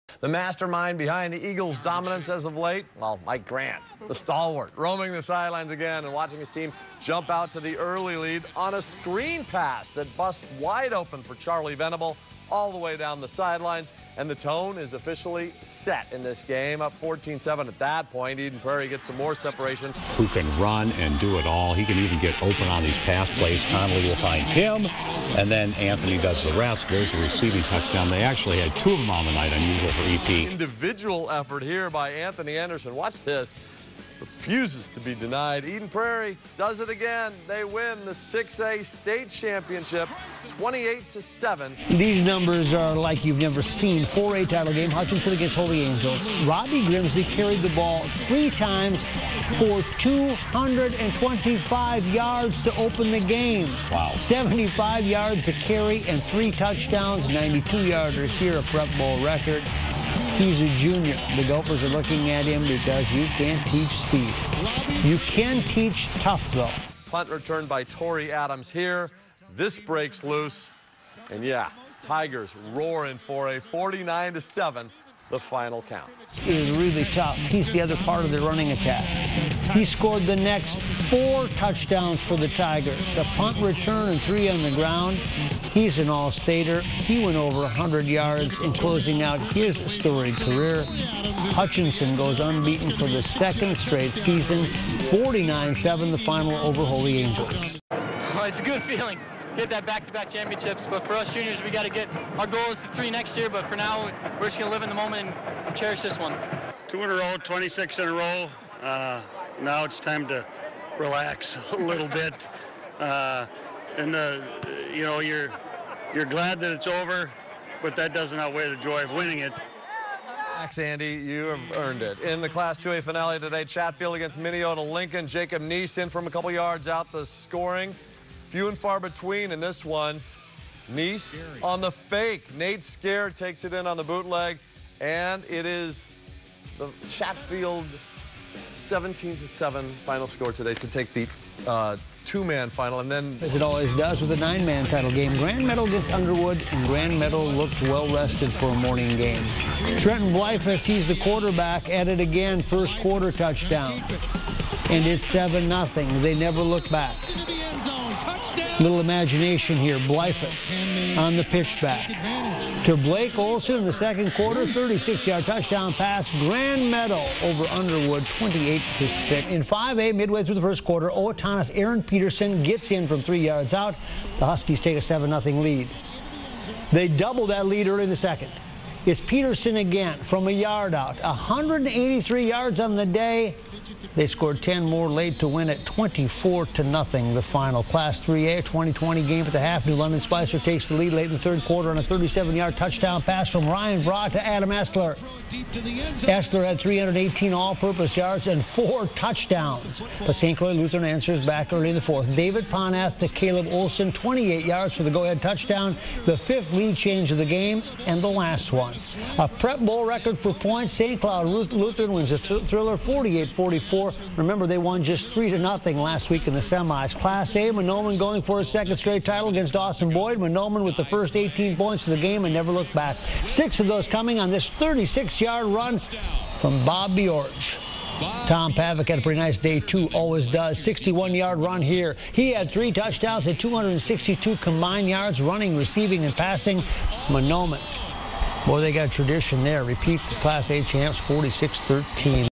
Prep Football: Playoff Highlights, Prep Bowls (2013)
Highlights of various Minnesota Prep Bowl football games from the 2013 season.